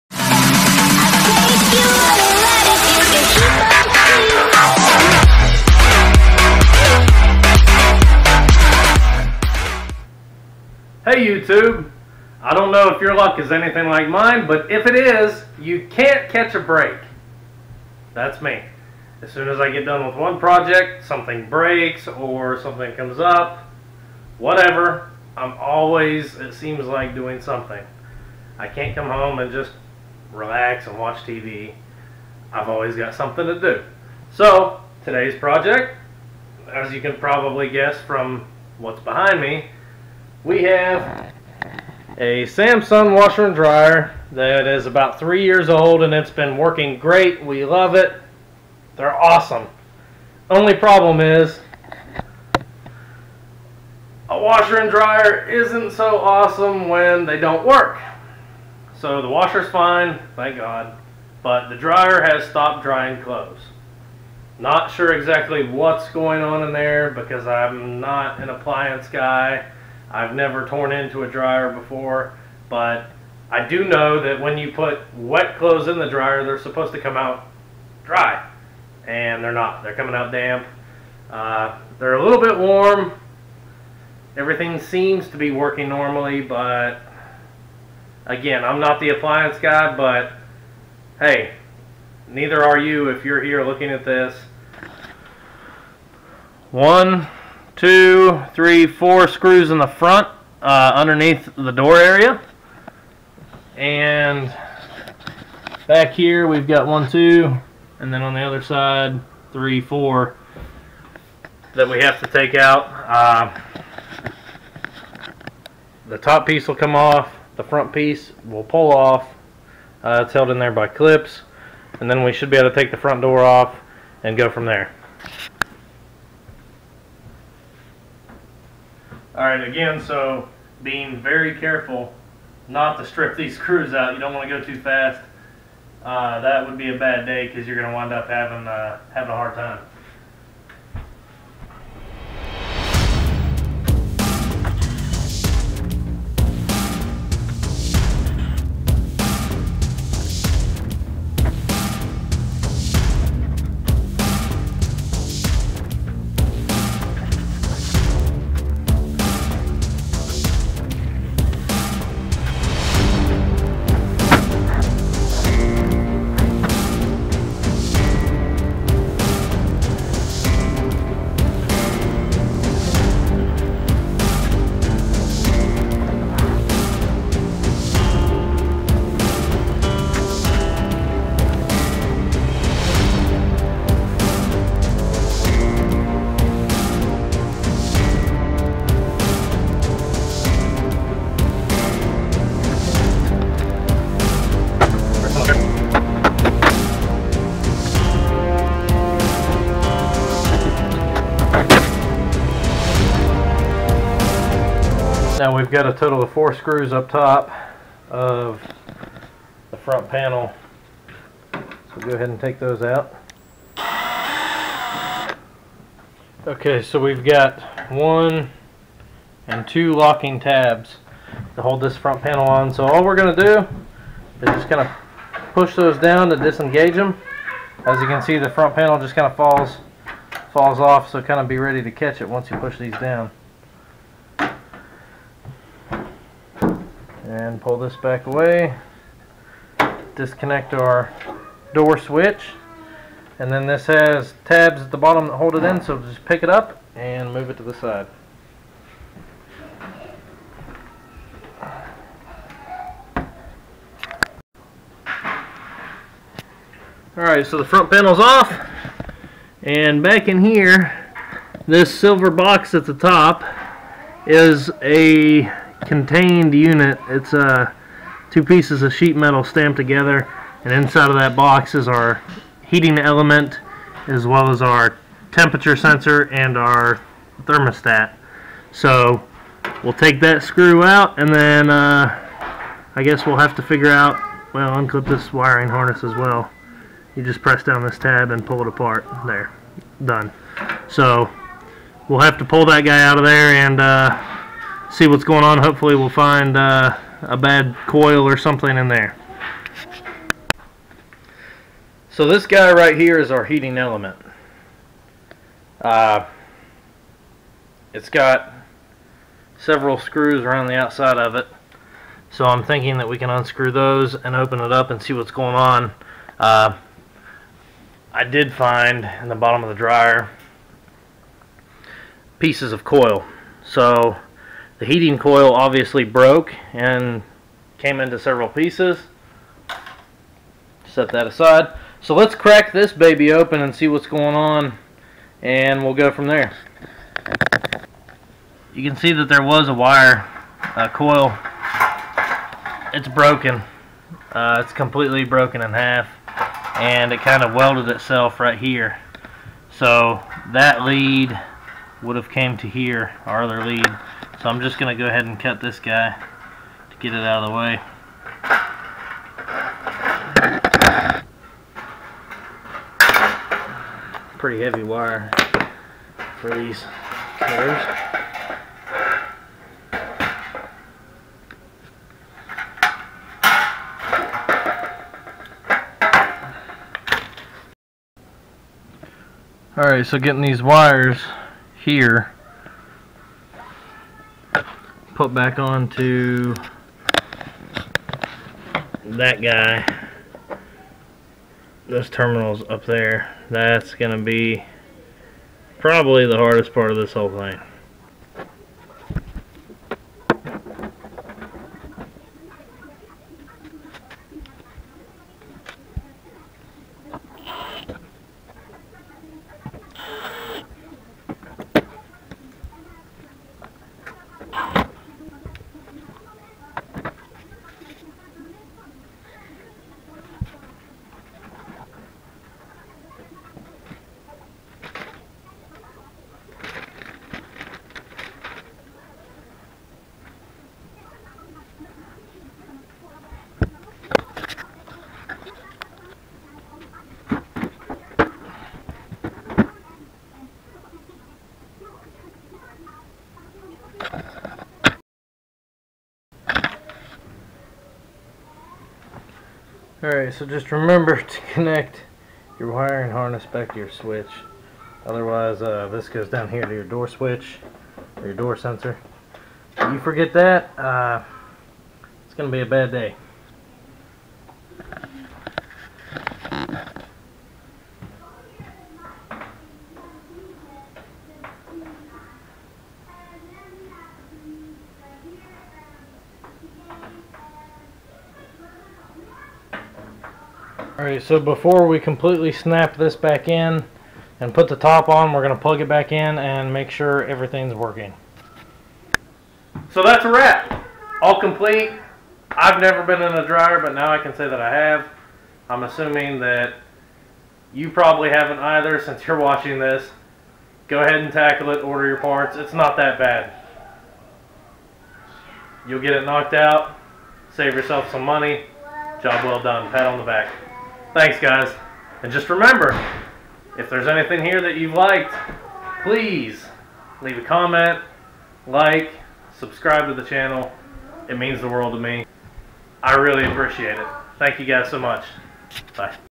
Samsung Dryer Heating Element
My Samsung Dryer stopped drying! Can we fix it?
Total cost to fix this dryer was $14
I purchased my part from Amazon, and had it in 2 days thanks to prime!

Heating element for DV42H5200 Samsung Dryer

Total working time: 1 hour